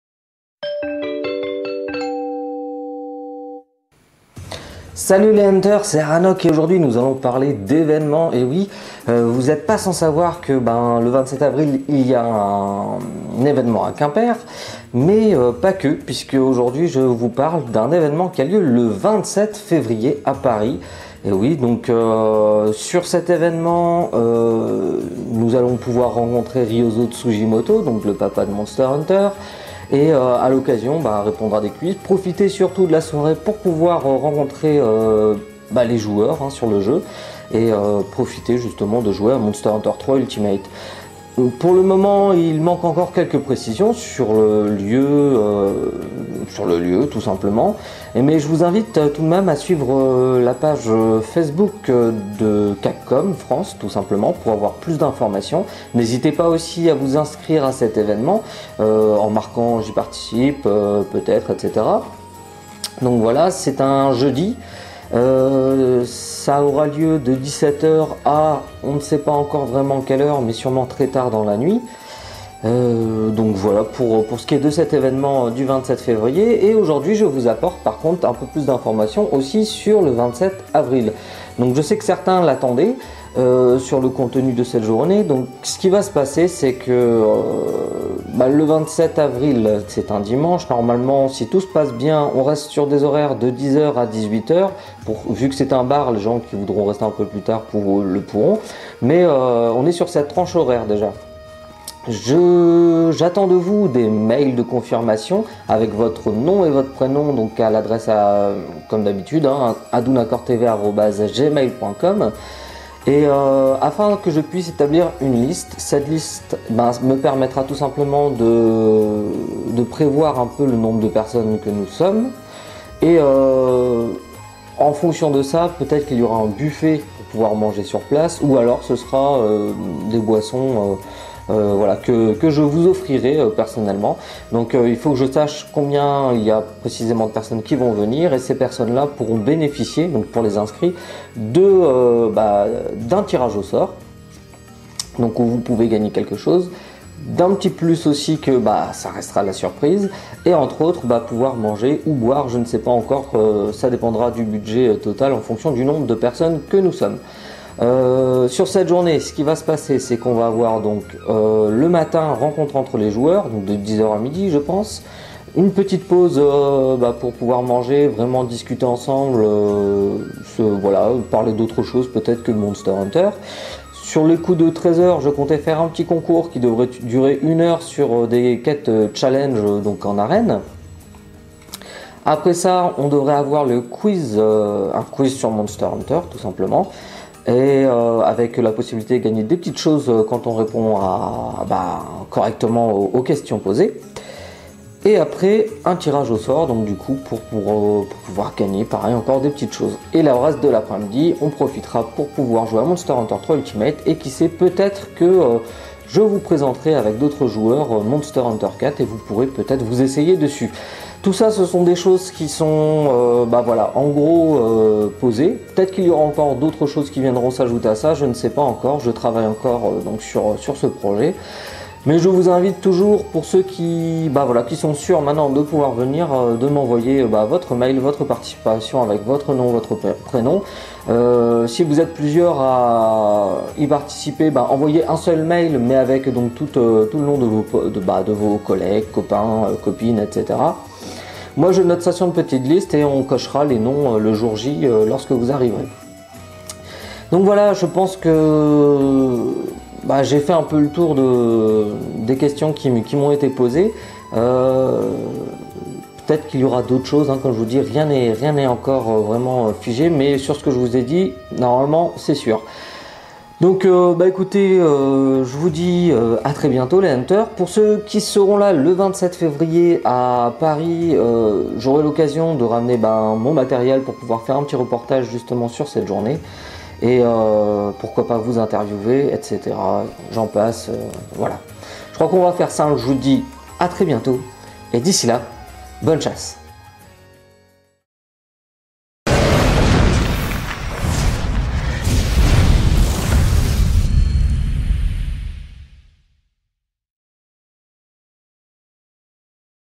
Evénement le 27 Février à Paris et le 27 Avril à Quimper
En attente d'information pour le lieu à paris.
Pour l'événement de Quimper il se déroulera le 27 Avril de 10h à 18h au bar Le café des arts 4 Rue Sainte-Catherine, 29000 Quimper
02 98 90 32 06